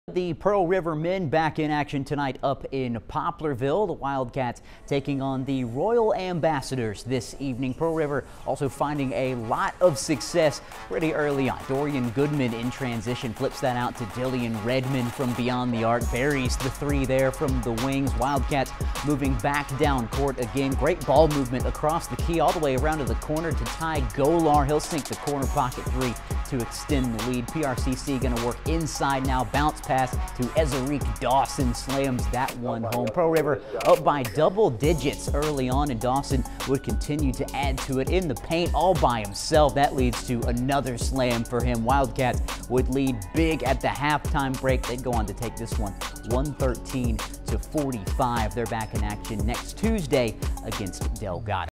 JUCO MEN'S BASKETBALL: PRCC vs. Royal Ambassadors (01/11/24)
Highlights from PRCC's win over Royal Ambassadors.